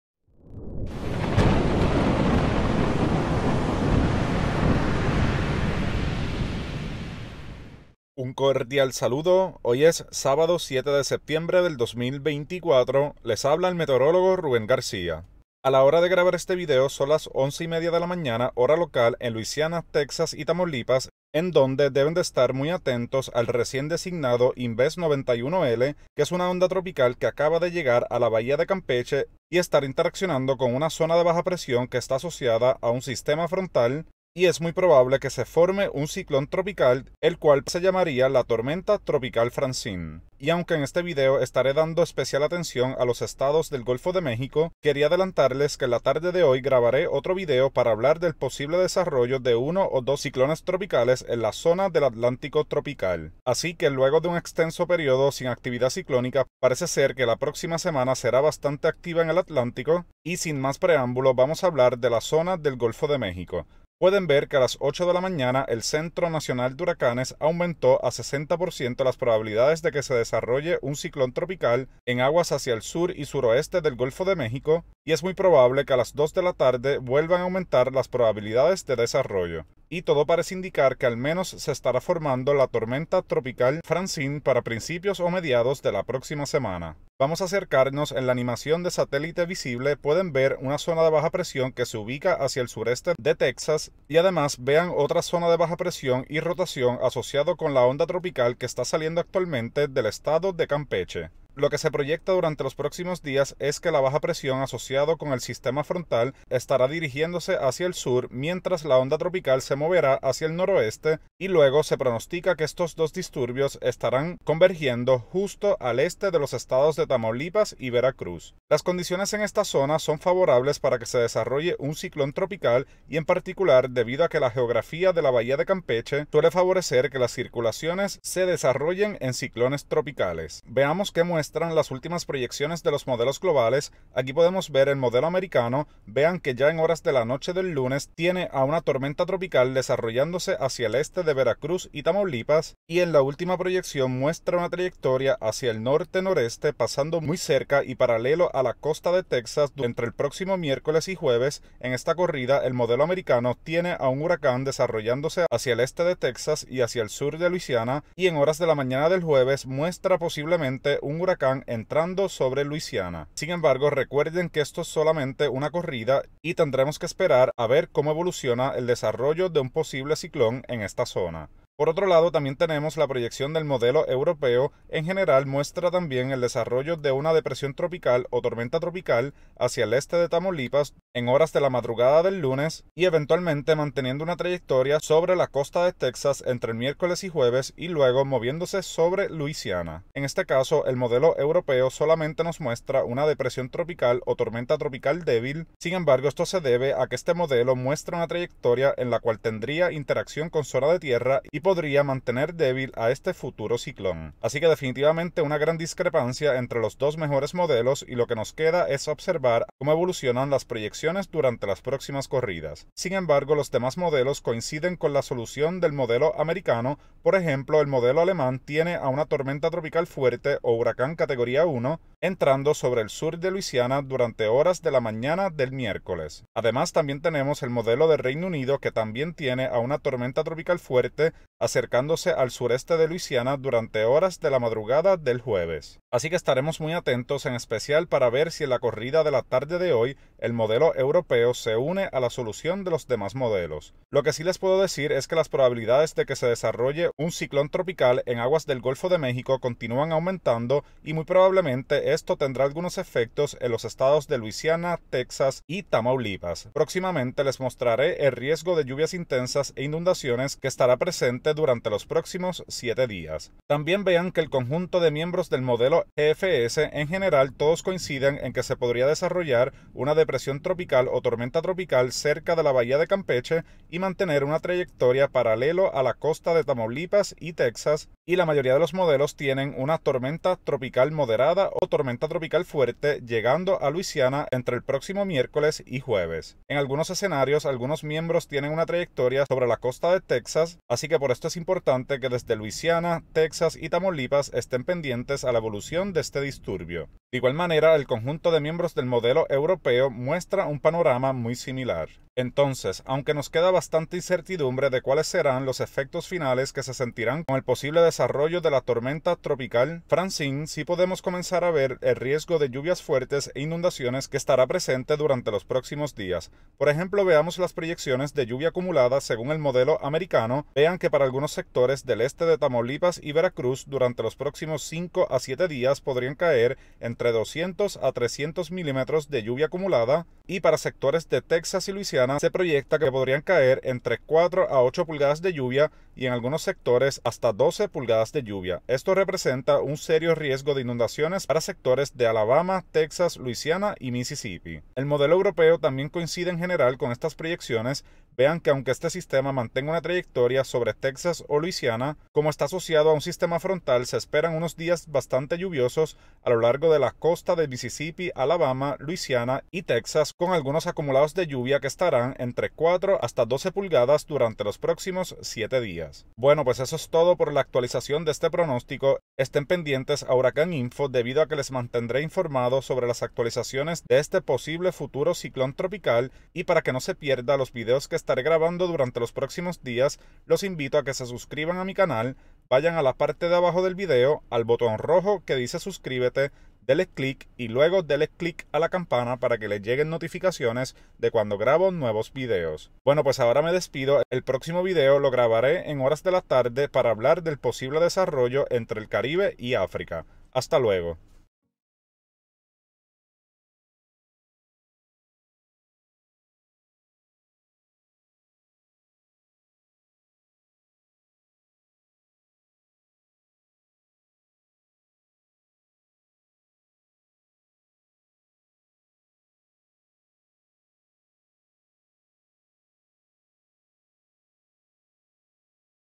Invest 91L (futura tormenta tropical Francine) amenaza a México (Tamaulipas), Texas y Luisiana.
Una onda tropical y zona de baja presión en el Golfo de México interaccionarán durante los próximos días. Las probabilidades de desarrollo de un ciclón continúan aumentando. Es posible que una depresión tropical o tormenta tropical se forme durante los próximos días cerca de la Bahía de Campeche.

Se proyecta que este sistema se moverá hacia el norte-noreste y afectará a sectores de Veracruz, Tamaulipas, Texas y Luisiana.